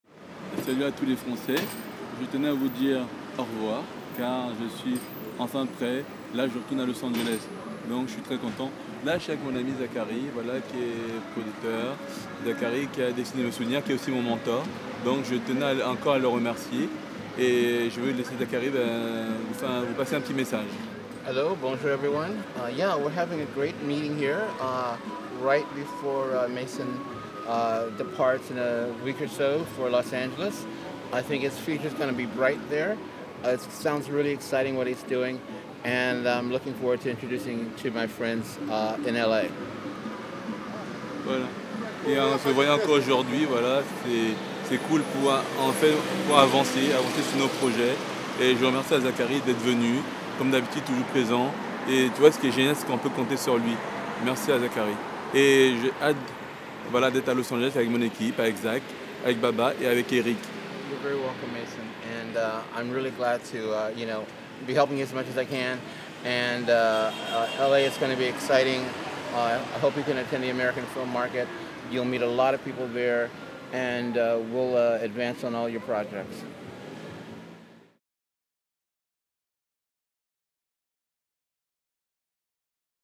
Mason Ewing supported by his mentor Zachary Miller - October 11, 2022 [subtitled]
Zachary Miller, American film producer, and former personal representative of Barack Obama in France, supports Mason Ewing in his film projects in the United States. Having a high-profile advisor will be highly beneficial.

Zachary Miller, représentant personnel de Barack Obama en France et producteur de cinéma, soutient Mason Ewing dans ses projets cinématographiques aux États-Unis. Ce dernier présente la manière dont il lui apporte son appui.